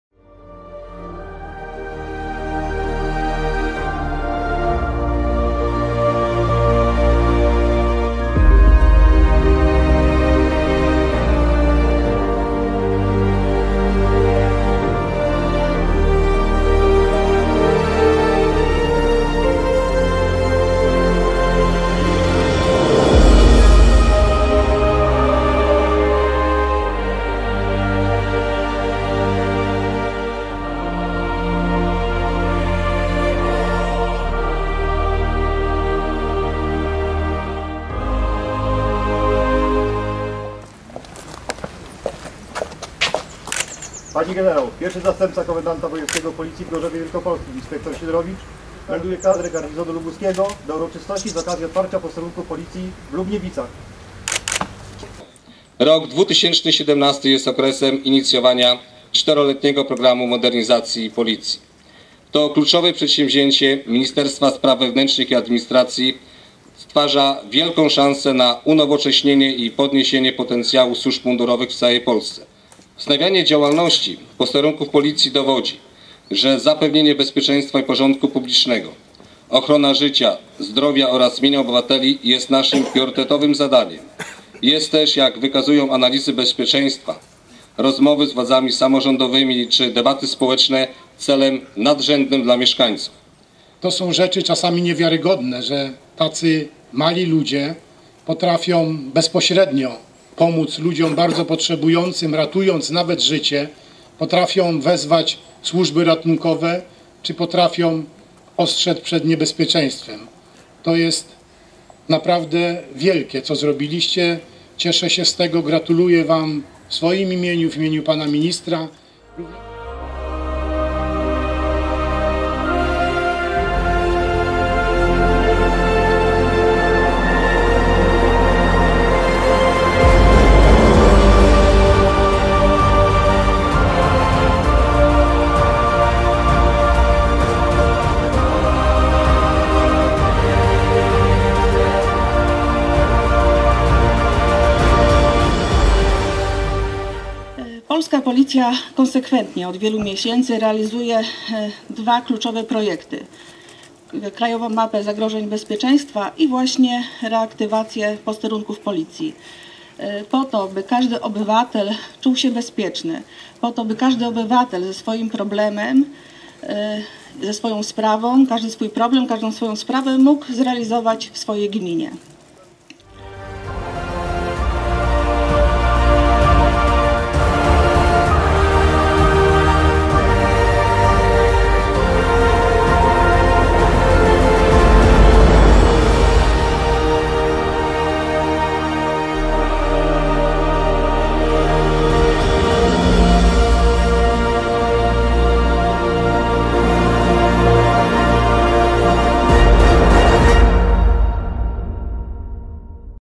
Przywrócony Posterunek Policji w Lubniewicach w służbie lokalnemu społeczeństwu
Garnizon lubuski powiększył się o kolejny, przywrócony posterunek Policji. To potrzeba, płynąca wprost od społeczeństwa, które sygnalizowało ją podczas licznych konsultacji i debat społecznych. W Lubniewicach Wojewoda Lubuski Władysław Dajczak, Zastępca Komendanta Głównego Policji, nadinsp. Helena Michalak, Komendant Wojewódzki Policji, insp. Jarosław Janiak oraz przedstawiciele władz samorządowych dokonali otwarcia posterunku.